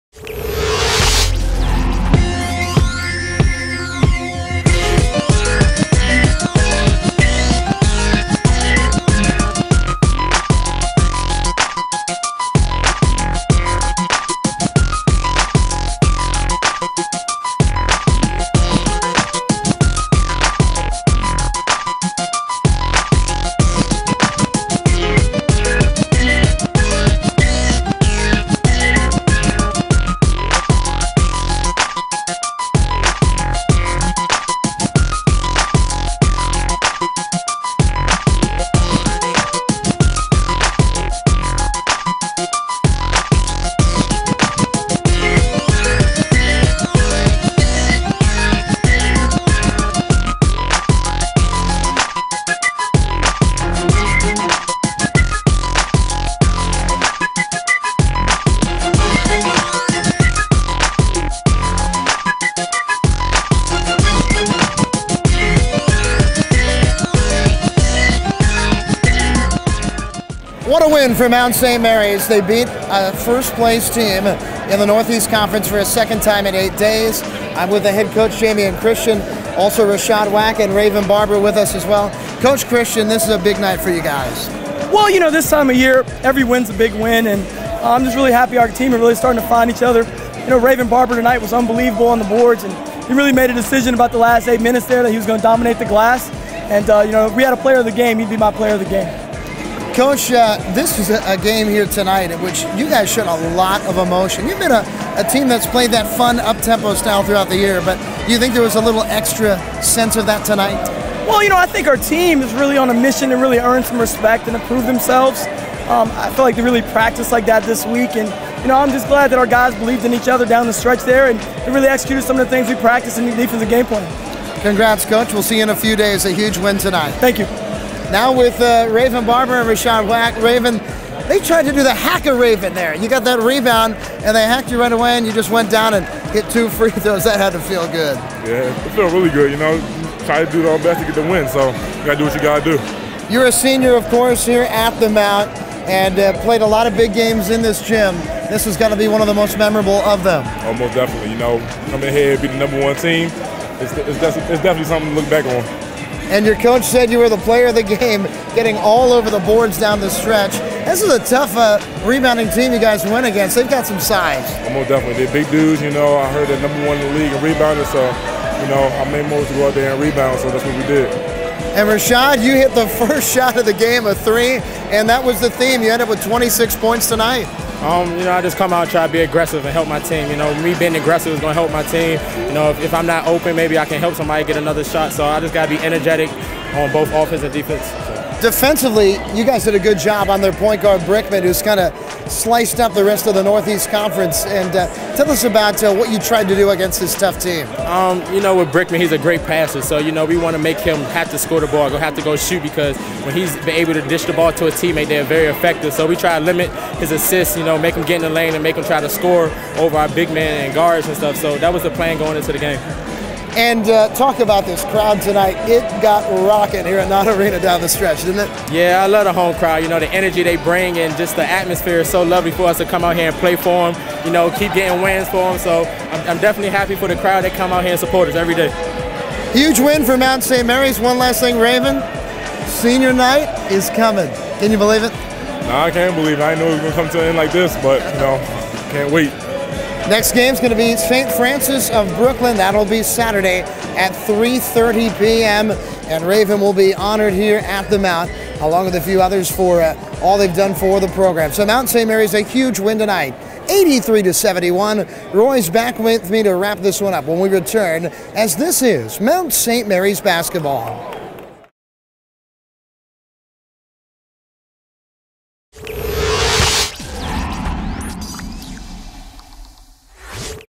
MBB Mount LIU
Highlights from the Mount's 83-71 victory over LIU Brooklyn